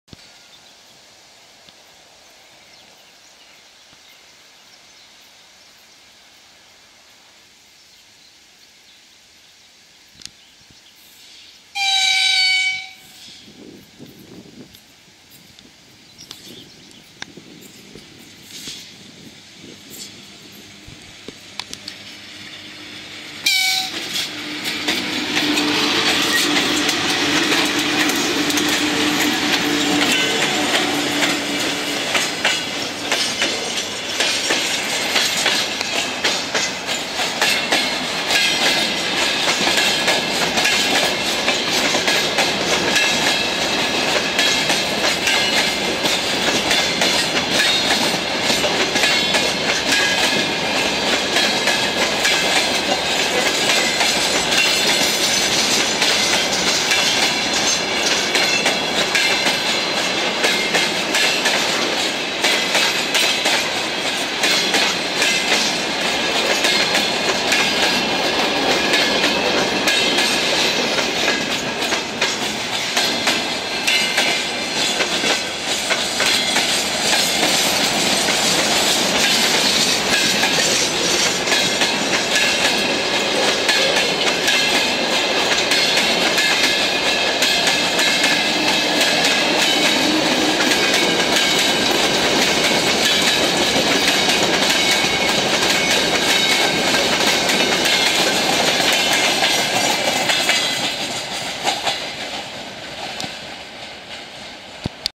Отправление электровоза ВЛ11М-349 с грузовым поездом со станции Белые Столбы, июль 2019 г.
ст. Белые Столбы.